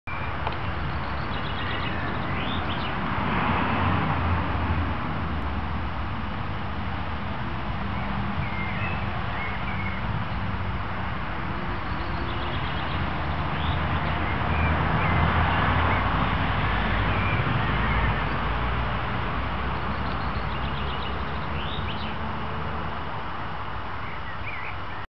Red Tailed Bumblebee Nest
A very short bit of video showing one RT bumble coming out and one, fully laden with pollen, going in.

Hidden on a grassy bank, it looks like the bees have made use of an old mouse hole for their nest.

It's actually the second time I've found a RT bumble nest at this site in my village of Corfe Mullen, Dorset, which makes it even more exciting.